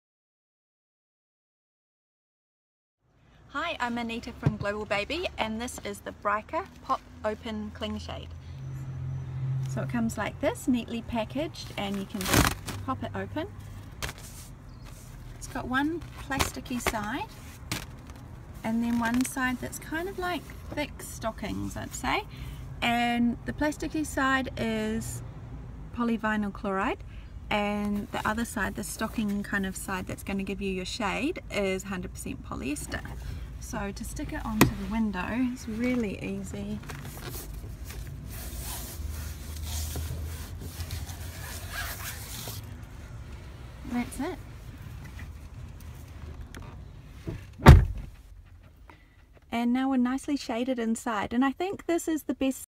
Brica Pop Open Cling Shade Demo
This is our favourite sun shade for the car. Simply stick it onto your window, no fuss, no mess, no sticky residue.

Anita from Global Baby